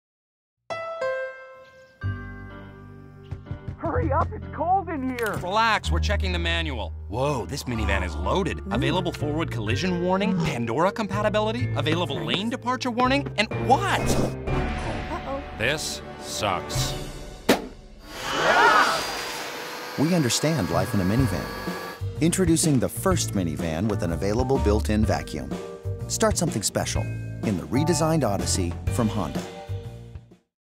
Honda Odyssey "Owner's Manual " Commercial
Introducing the first minivan with a vacuum